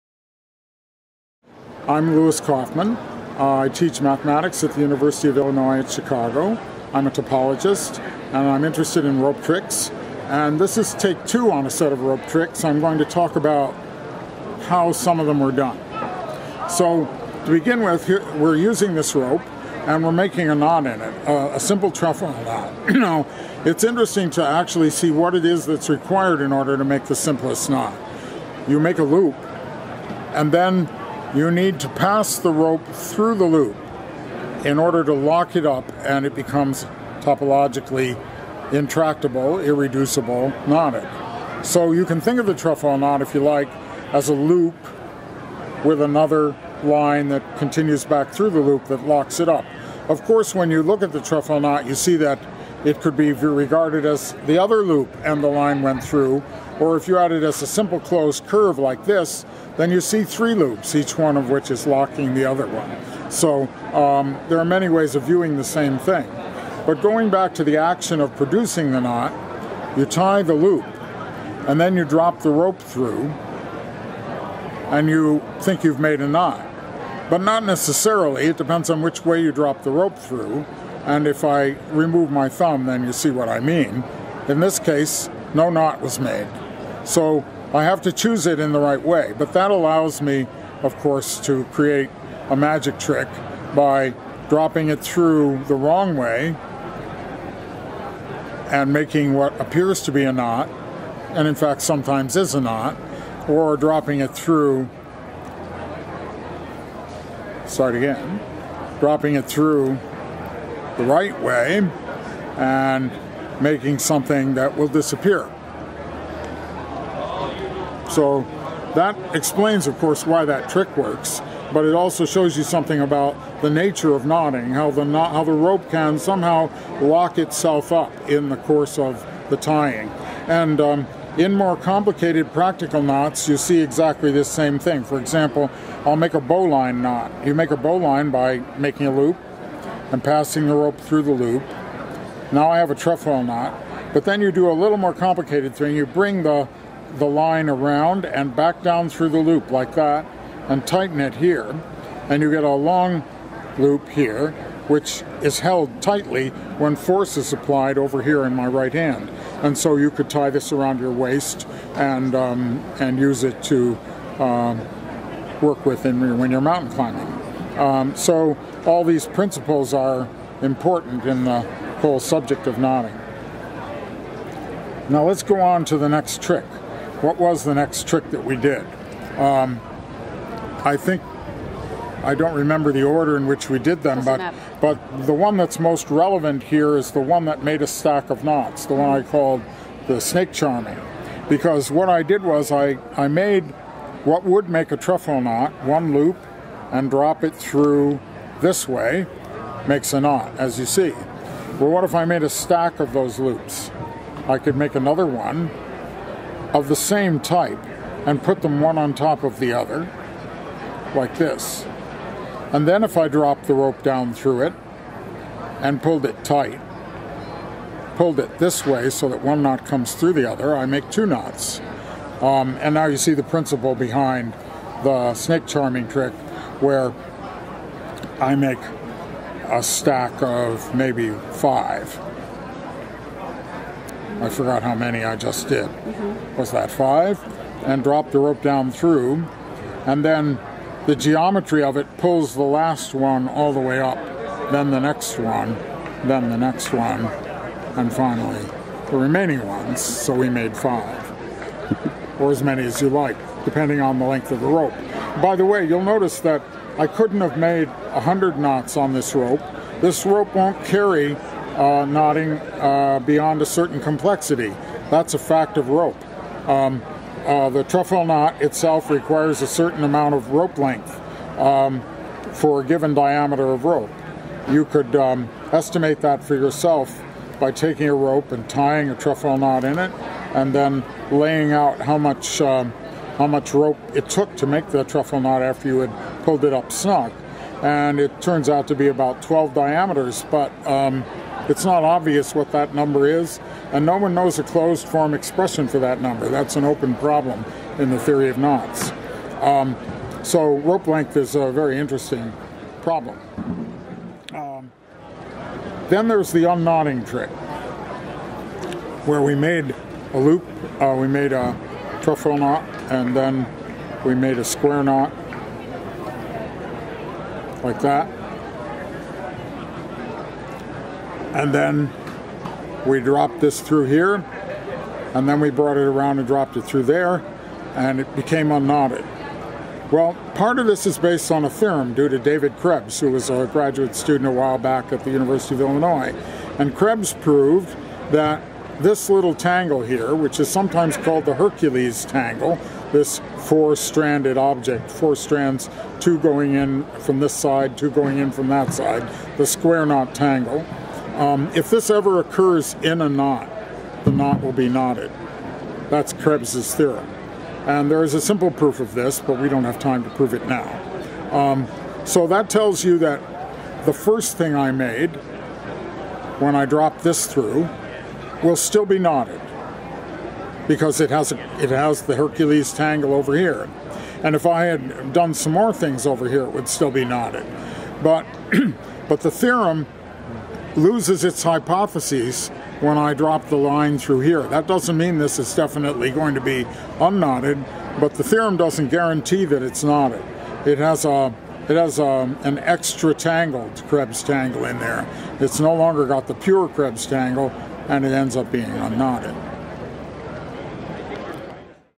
Louis H. Kauffman - Rope Tricks, part2
This video features Louis Kauffman, and was produced for Mathematics Awareness Month 2014. Interview by Bruce and Eve Torrence, filmed March 21, 2014. for more.